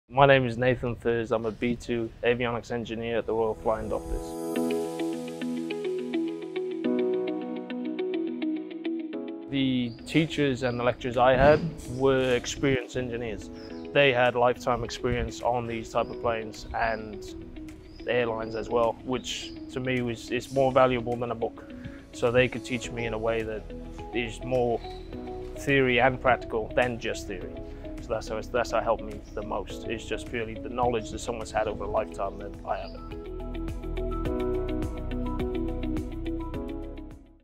General Aviation Career options | Aviation Australia
Hear from an Aircraft Maintenance Engineer working for the Royal Flying Doctors how his training has helped him in his career and how learning from teachers who have real world industry experience makes a difference.